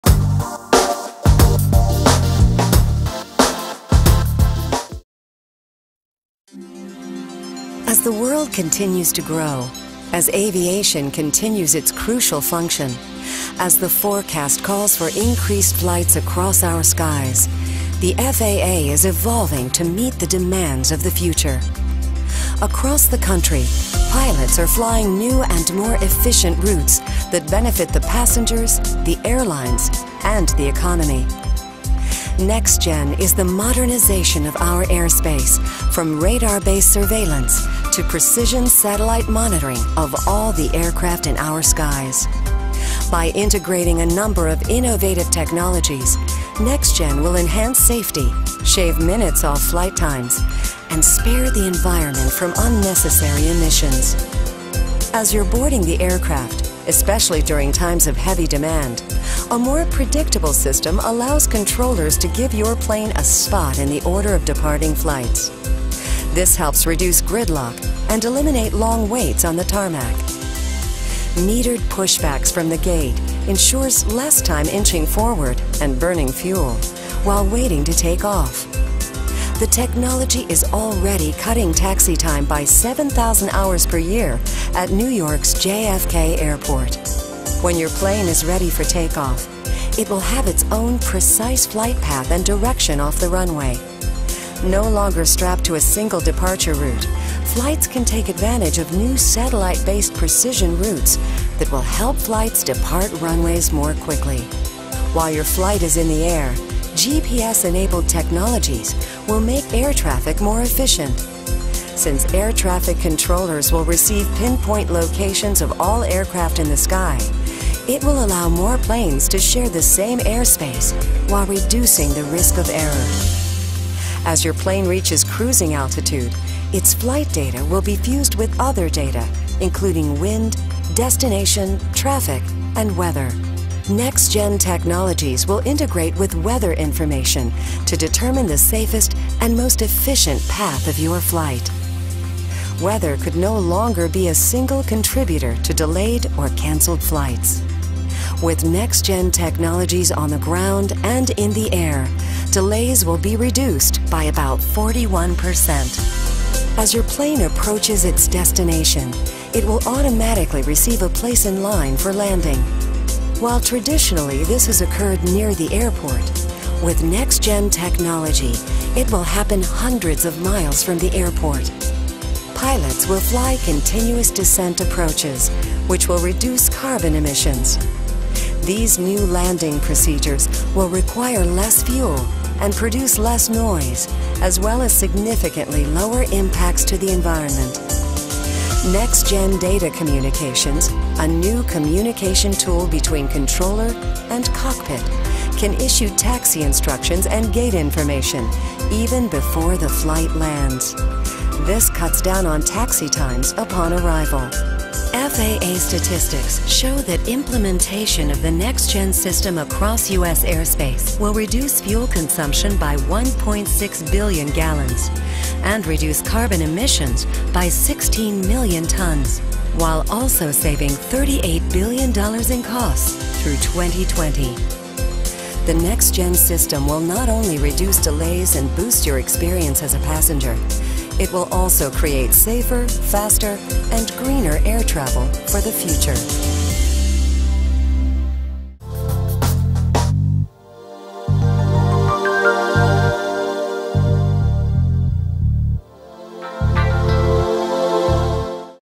NextGen Aviation
SLICEnews is ready for new ways to fly, aren't you? The Federal Aviation Administration is finally getting with the new millennium and changing the technology that currently makes air travel miserable. By using fresh tech a promise of safer, faster and greener travel is pending.

Let us know what you think with the SLICE APP